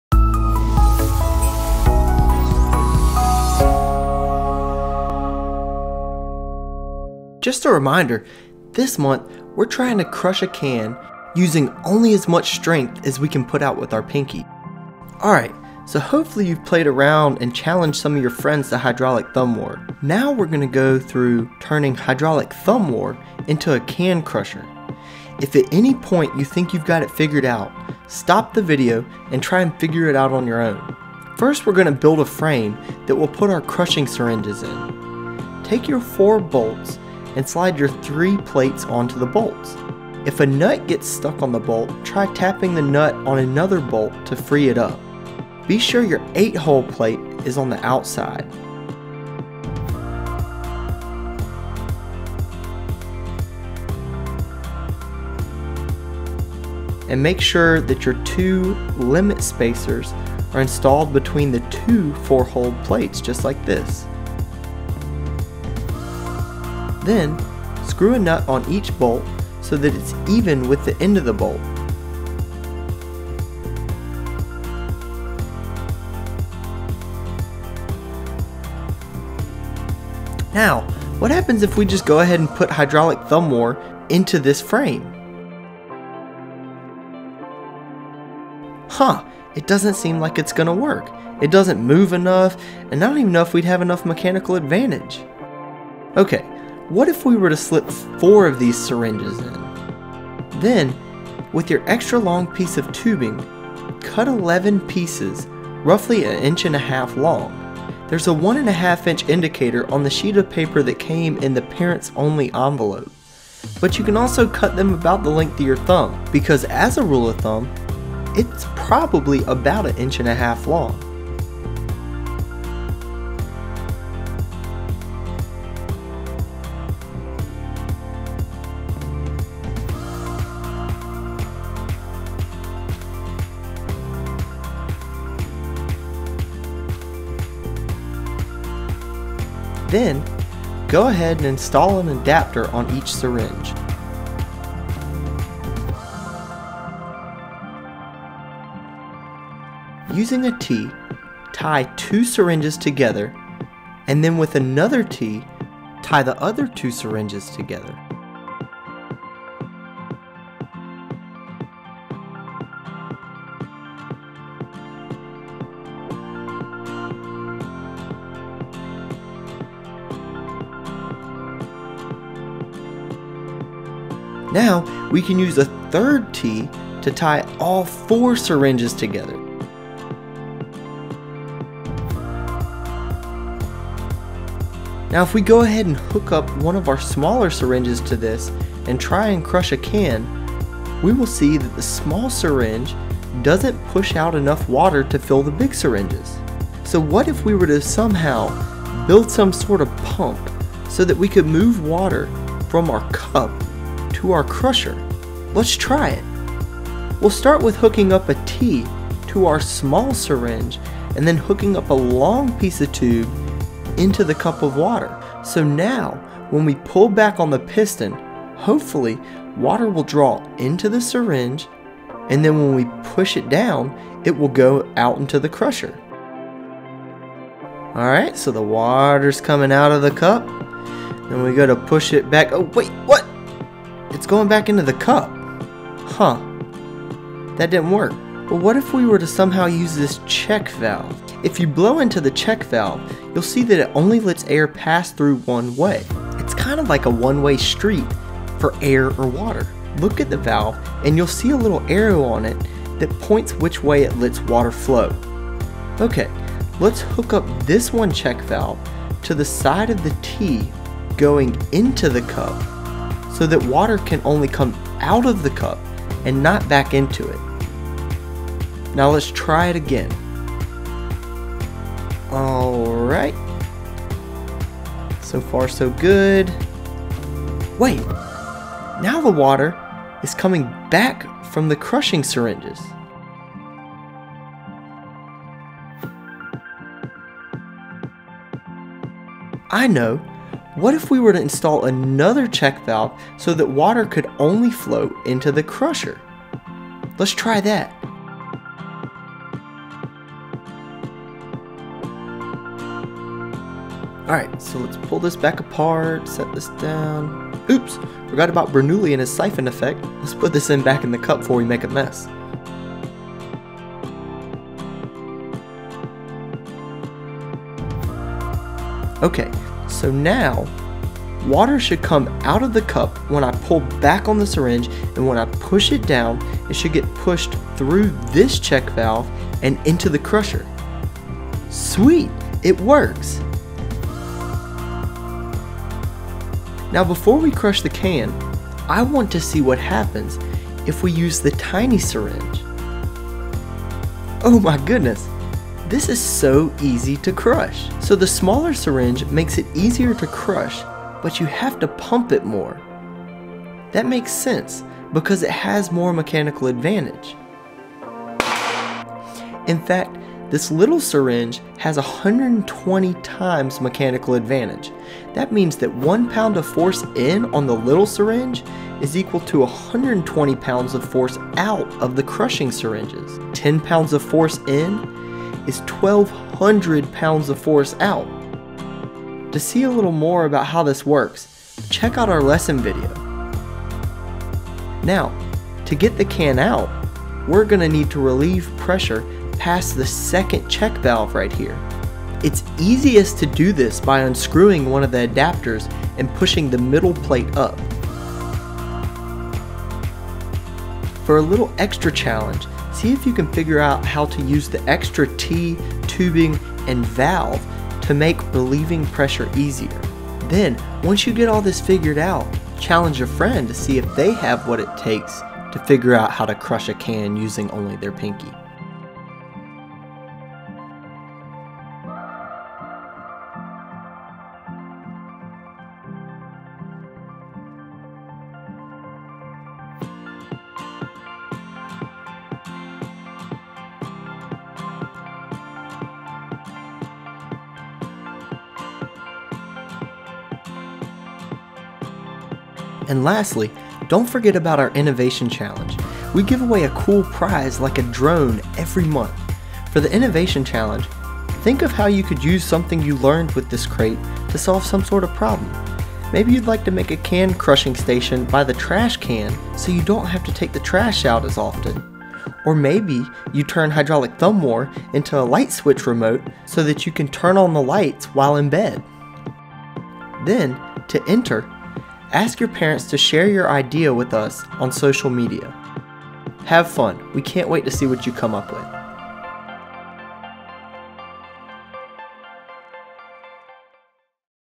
How to Make a Hydraulic Can Crusher with RumbleLab
This DIY Can Crusher allows you to crush cans using only your pinky!

Get the parts to invent this yourself with our Mission To Mars challenge crate.

On our Mission to Mars, problems came up and as engineers, we had to figure out how to solve them. One of the problems was, we needed a way to crush cans. In space there is no gravity, so stomping on cans is out of the question. We want to see if you can figure out how to invent one using the parts from your Mars crate.

This crusher has a mechanical advantage of 40x. It has a maximum crushing force of 400 lb. This crate teaches about mechanical advantage and Pascal's law.